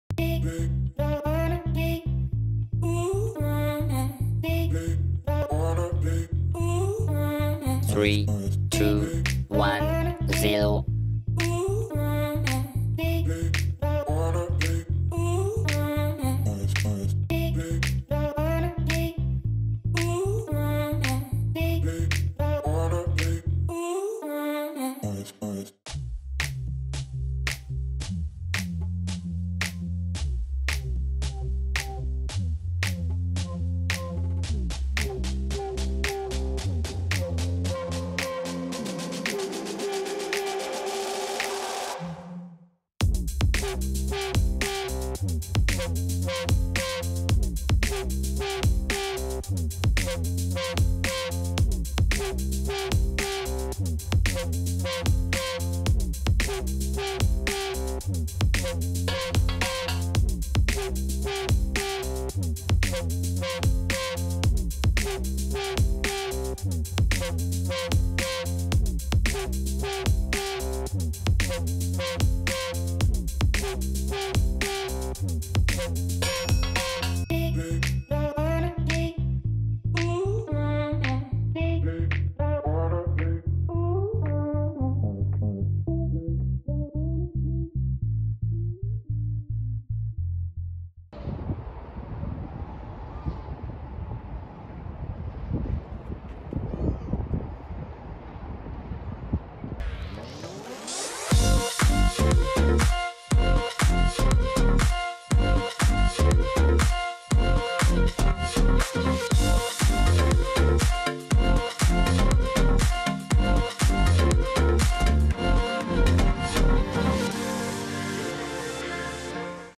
【○○をさがせ！】スカイツリーのふもとで、シカがにげたぞーーー！
【○○をさがせ！：東京スカイツリー編】
がめんのどこかにシカがかくれています。
みつけてください。

どちらかというと、シカです。